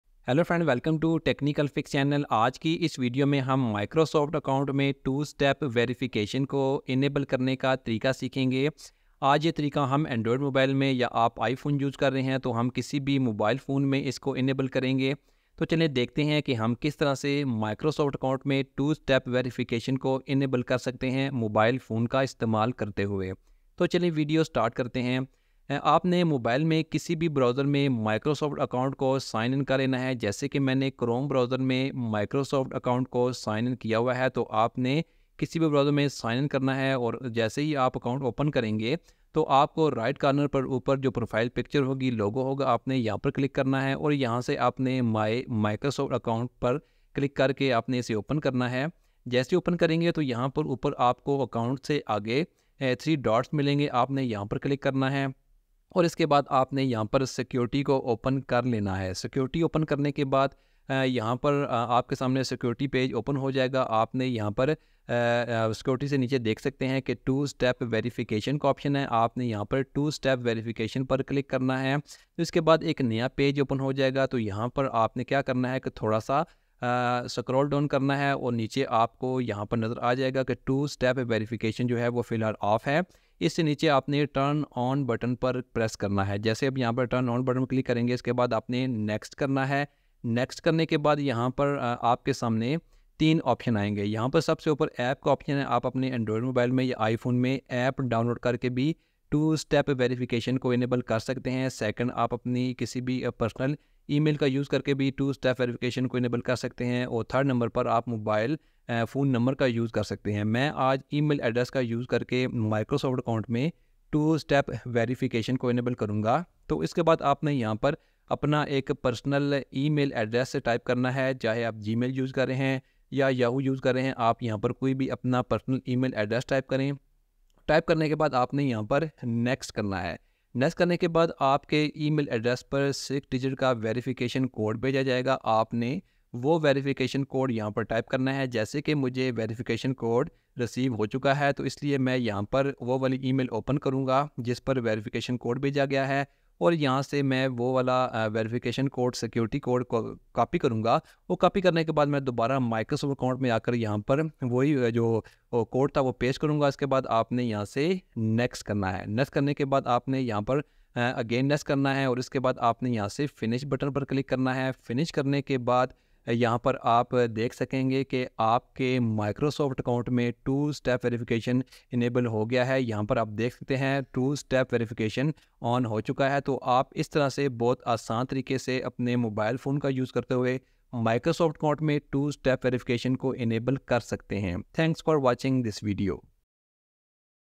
Turn on Two Step Verification Microsoft Account on Android/iPhone
In this video we have learned how we can turn on the Microsoft account two step verification inside the mobile phone.

Cover Topic:
1: microsoft two step verification on
2: two step verification on outlook
3: two step verification on live